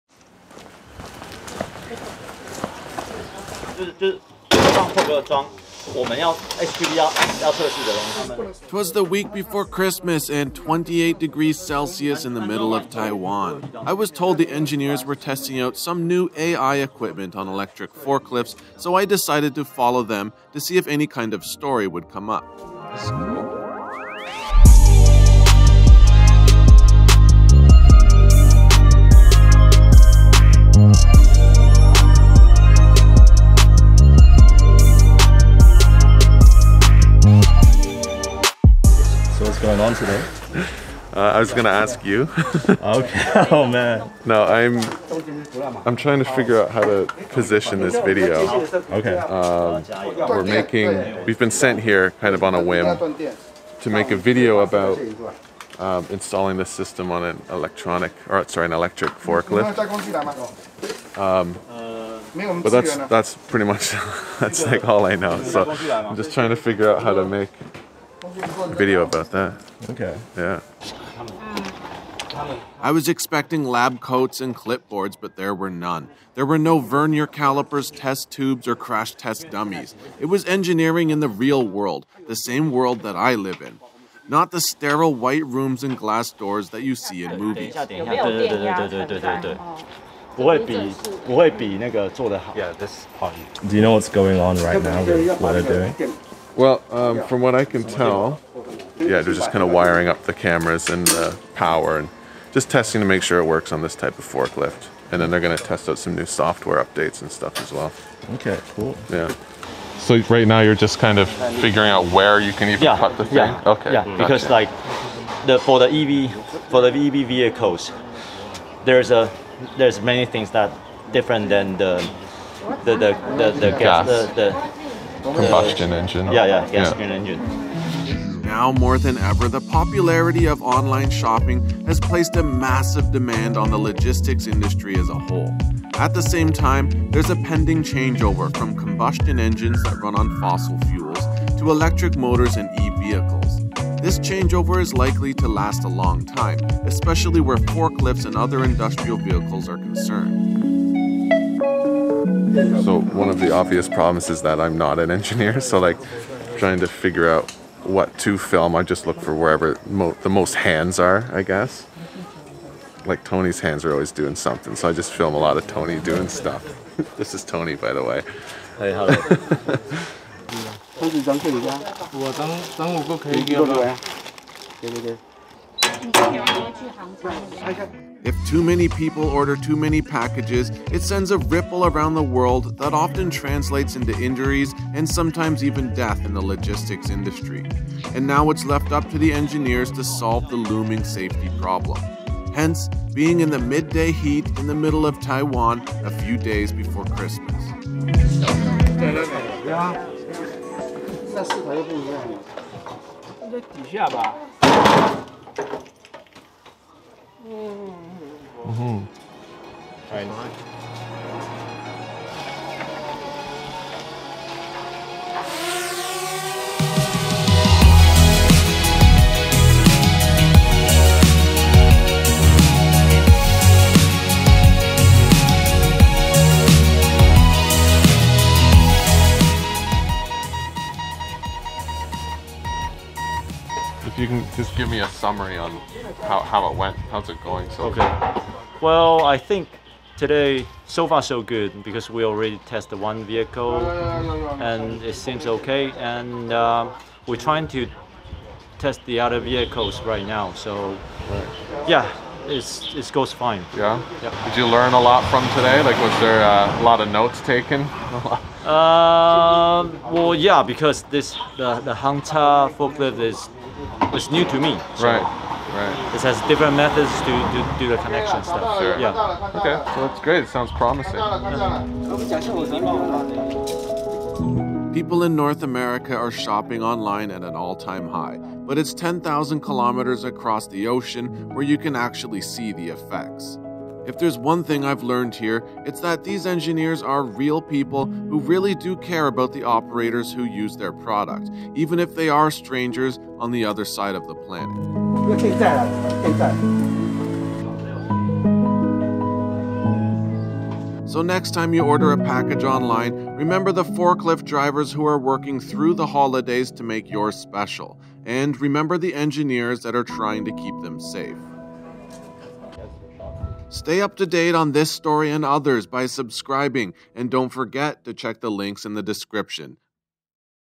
Installing the VIA Mobile360 Forklift Safety System on EV Forklifts
In this episode, we are following a team of engineers to the middle of Taiwan where they plan to test out our VIA Mobile360 Forklift Safety System on electric forklifts!

To learn more about this innovative pedestrian detection system, check out